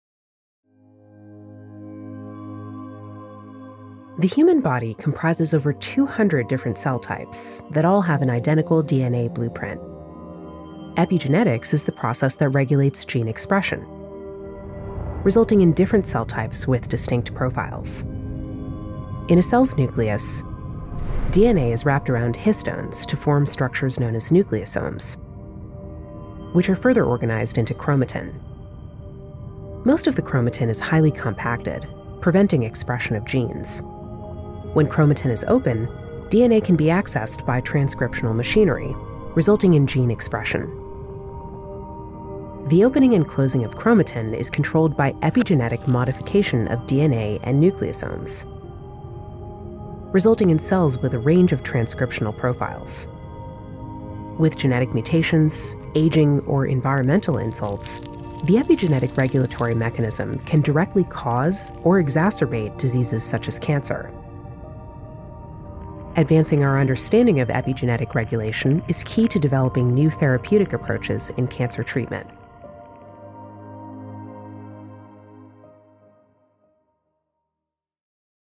Epigenetics: the next revolution in cancer treatments
In order to expand our portfolio, we are investing in technology to better understand the role of epigenetics in tumours. Our goal is to advance our understanding of epigenetic regulation to identify new therapeutic approaches to improve cancer treatments. Learn how genetic mutations and environmental insults can cause cancer